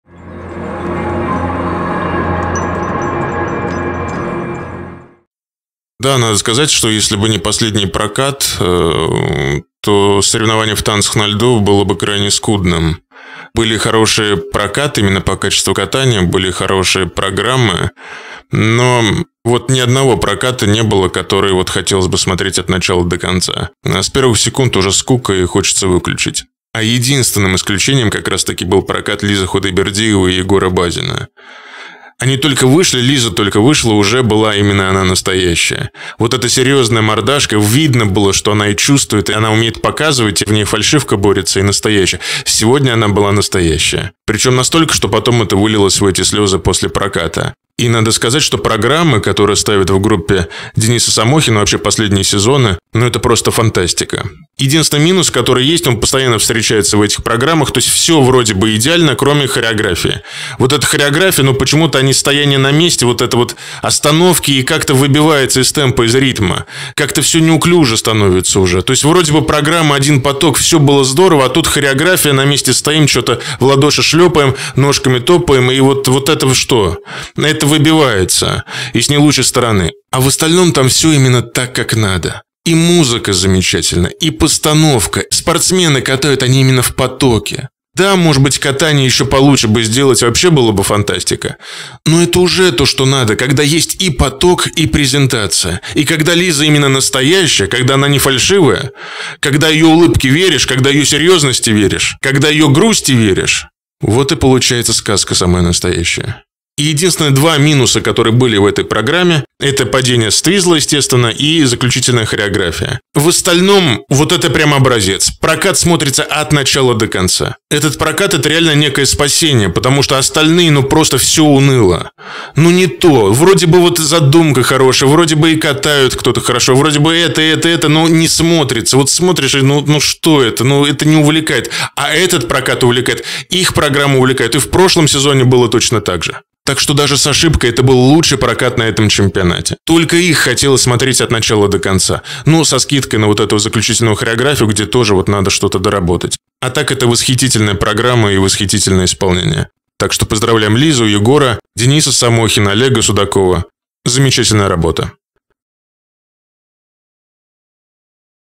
ВСЁ РАВНО ШИКАРНО! Елизавета Худайбердиева / Егор Базин. Чемпионат России 2023
Елизавета Худайбердиева / Егор Базин, произвольный танец, чемпионат России по фигурному катанию 2023 в Красноярске. Все результаты

Елизавета Худайбердиева и Егор Базин стали чемпионами России по фигурному катанию в соревновании танцев на льду. Тренируют дуэт Денис Самохин и Олег Судаков.